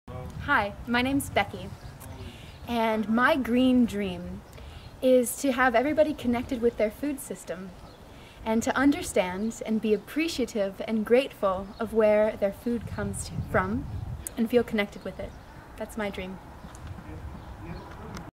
Connection to food system
Everybody should be connected to their food system and understand and appreciate what goes into food production.